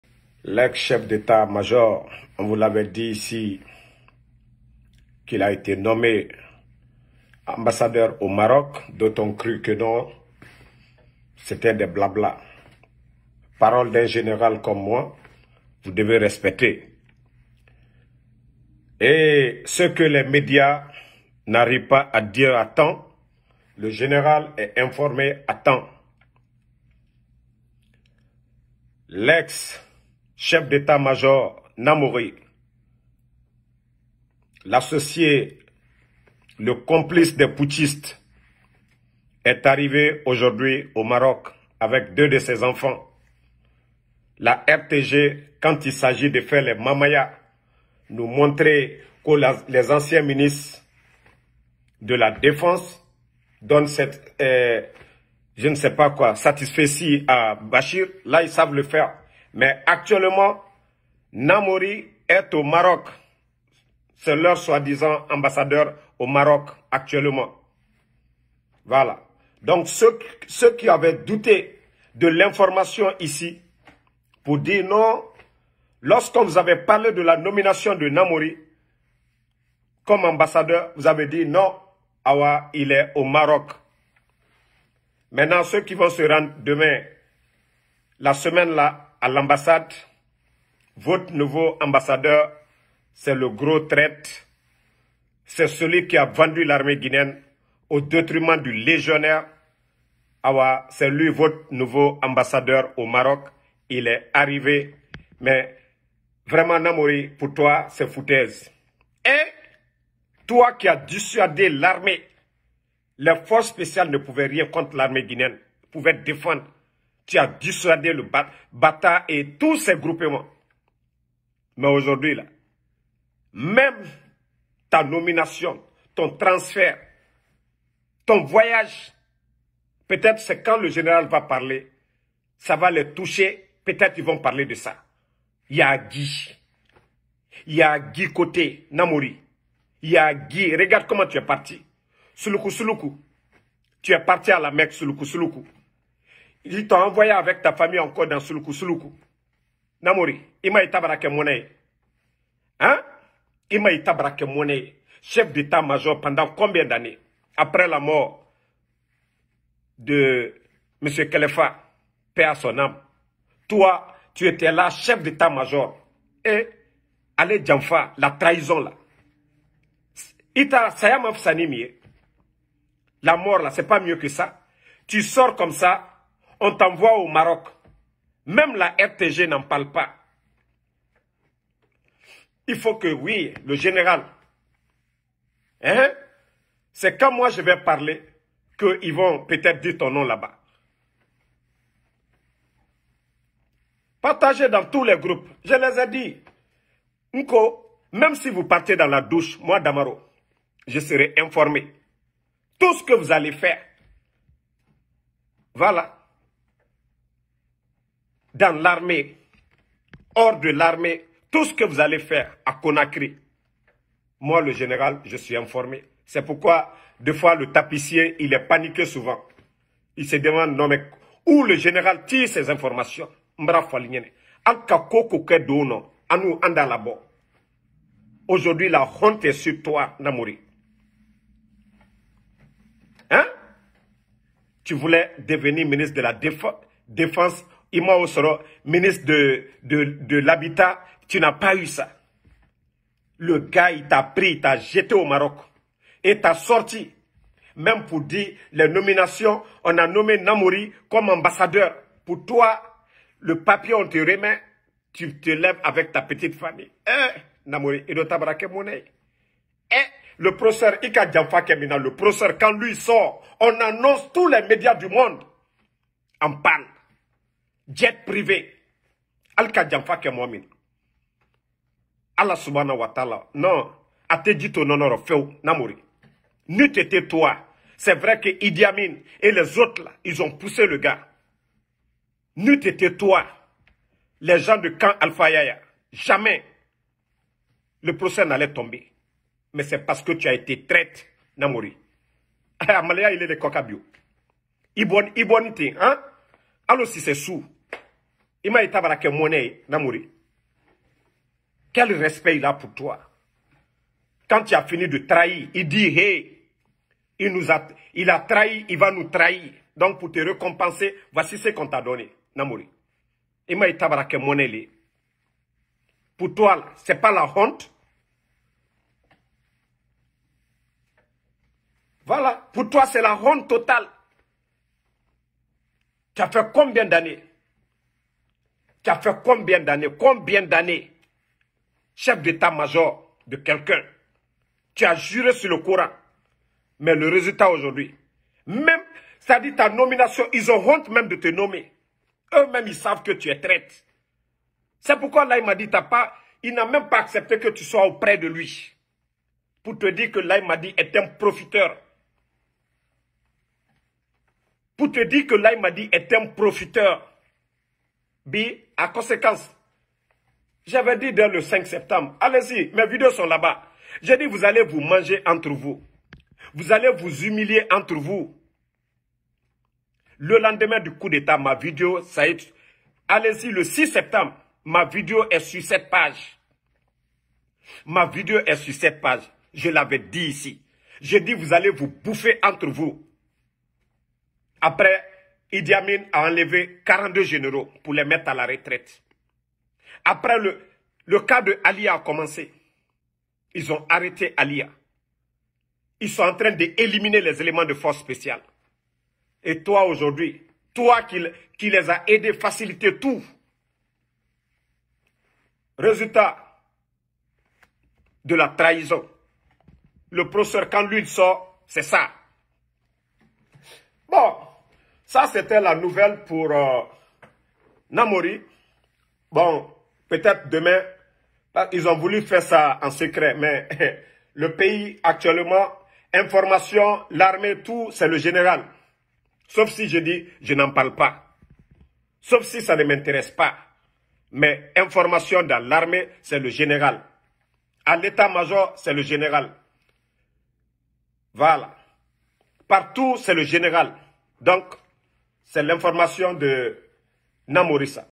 GÉNÉRAL NAMORY LE TRAÎTRE EST NOUVEAU AMBASSADEUR DE LA GUINÉE AU MAROC
GÉNÉRAL NAMORY LE TRAÎTRE EST NOUVEAU